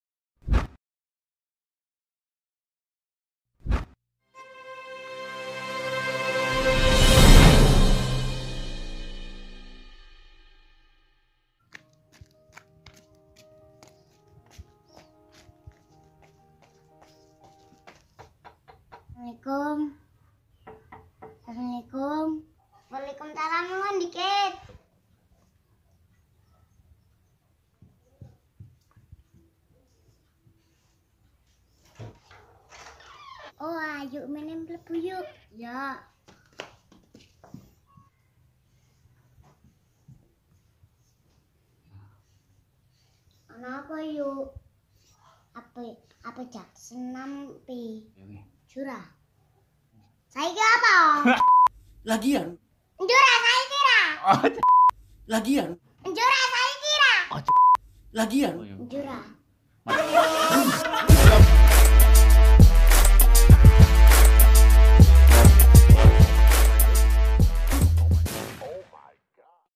Ngajak Senam (Film Pendek) bersama Coki Muslim MLI
film pendek "Ngajak Senam" ayu seorang anak yg datang kerumah sahabatnya vivi untuk mengajak senam yg tiba2 muncul coki dan muslim mli, ikut meramaikan video film pendek Ngajak Senam. tonton sampai habis keseruannya.